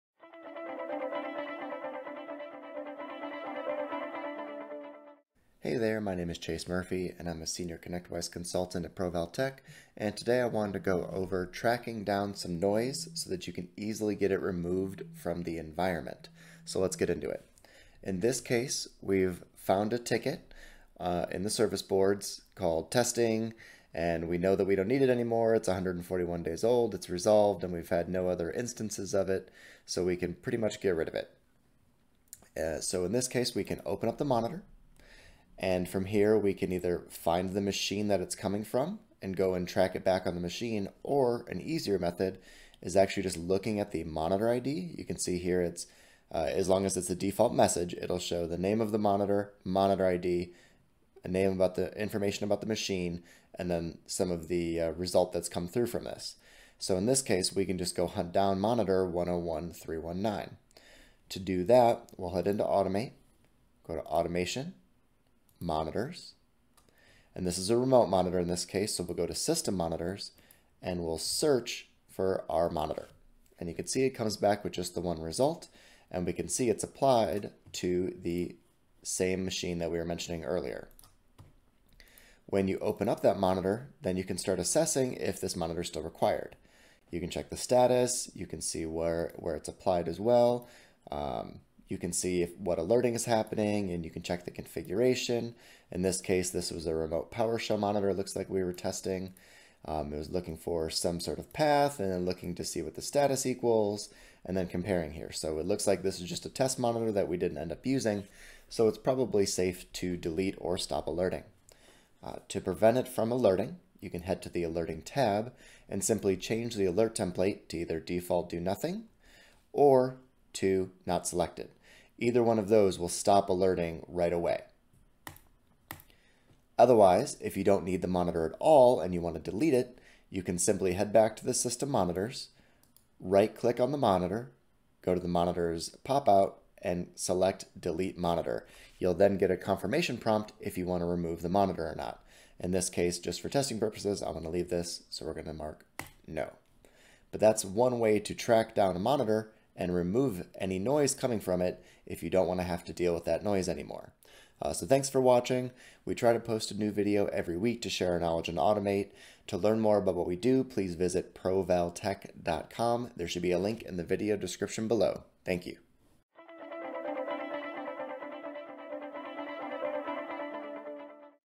ConnectWise Automate: Taming the Noise
Automate can be noisy out of the box! Watch this video if you want to learn how to track these monitors down and shut them off. In this particular video we will be tracking down a system based (remote) monitor, and then showing you how to remove the monitor or disable the alerting.